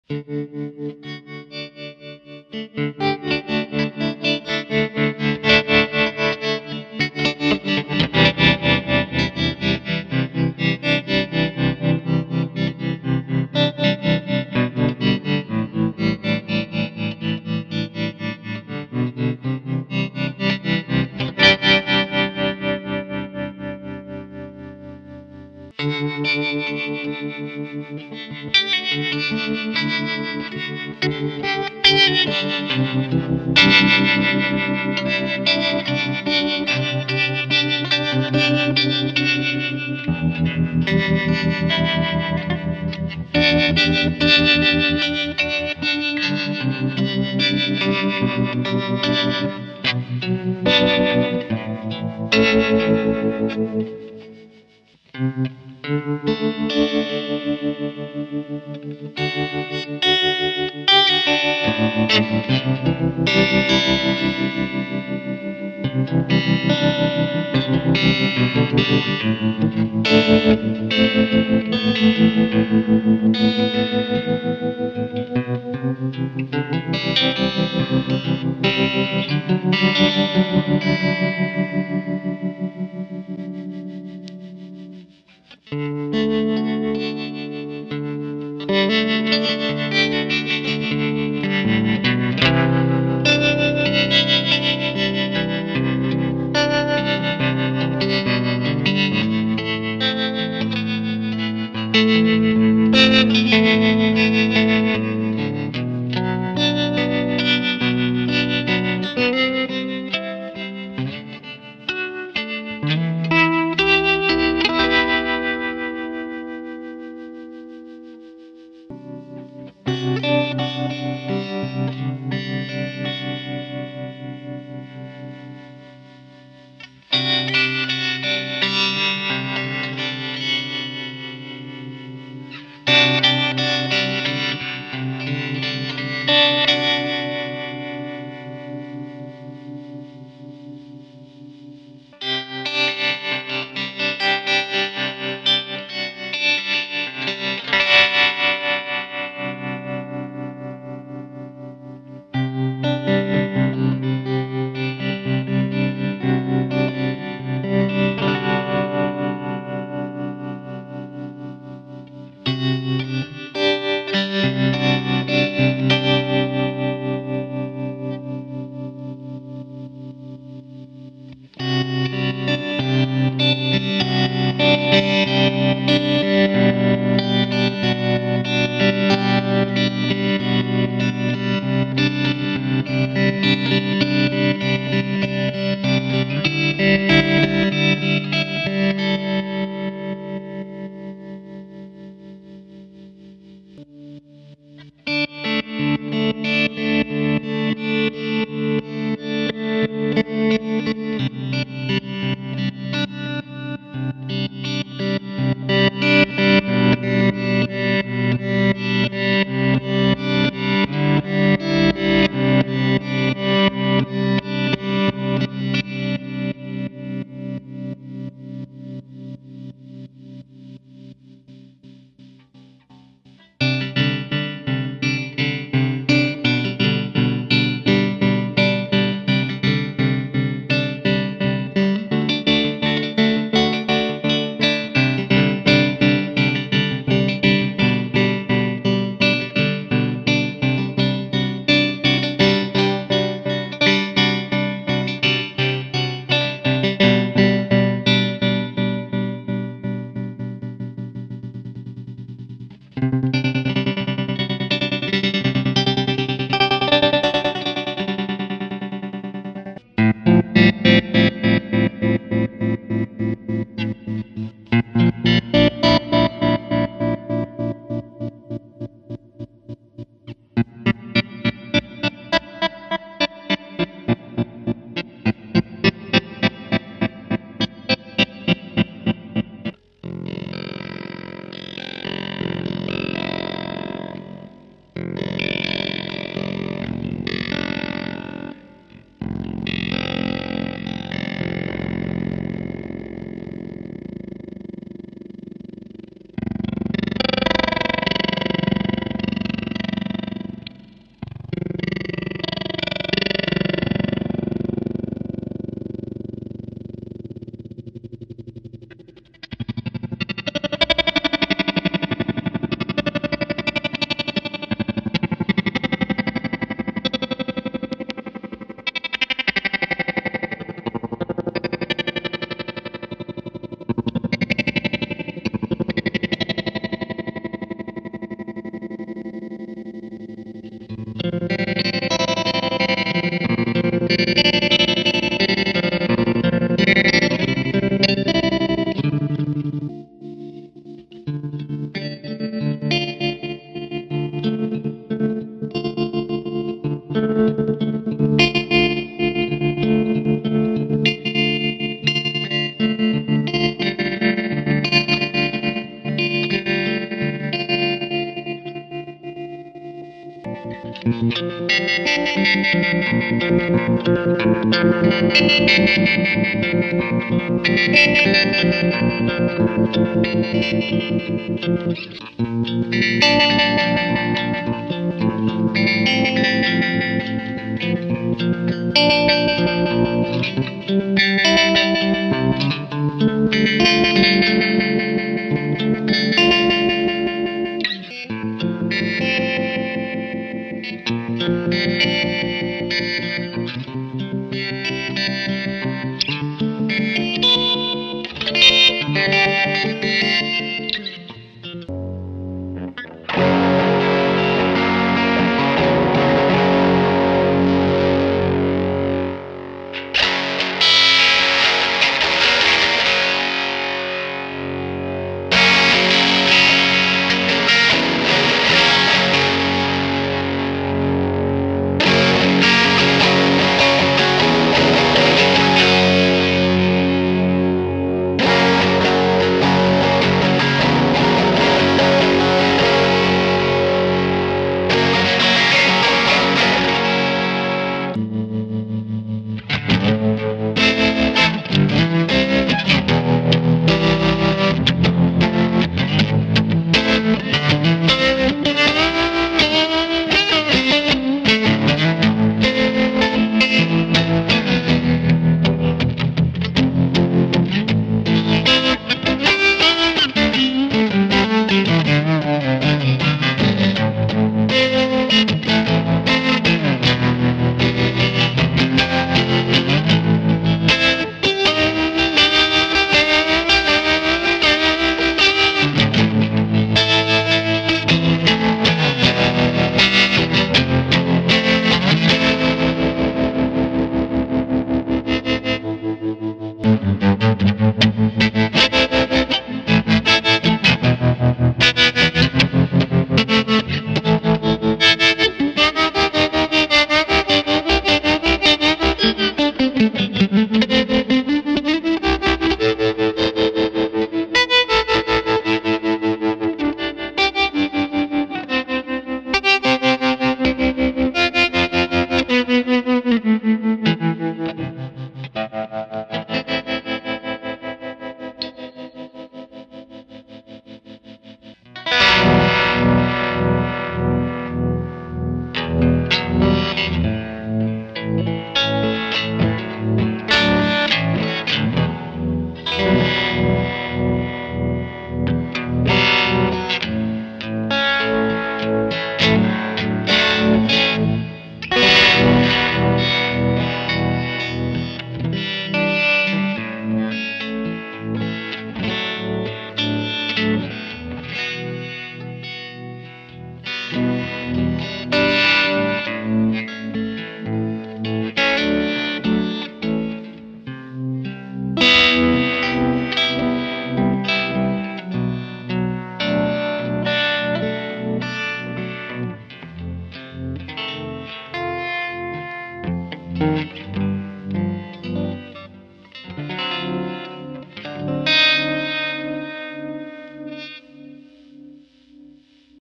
drolo - Twin Peaks Tap Tempo Tremolo / Harmonic Vibrato / Multiple Wave Shapes
The Twin Peaks is a tap tempo tremolo with 4 modes:

Classic ( like Black Face tremolo ,regular volume modulation)
Harmonic (like the Harmonic Vibrato in old Fender Brown Face amps, pans between treble and bass)
Bass Only (Bass modulation only, with fixed treble)
Treble Only (Treble modulation only, with fix bass)

The LFO has 8 available modulation shapes:
Sawtooth, Reversed Sawtooth, Square, Triangle, Sine, Lump, Reverse Lump, Random

Controls:
Rate, Division, Tone, Depth, Shape, Shape distortion

While the Tap tempo LFO section is digital, the audio path is 100% analog. The tremolo effect is achieved using optocouplers.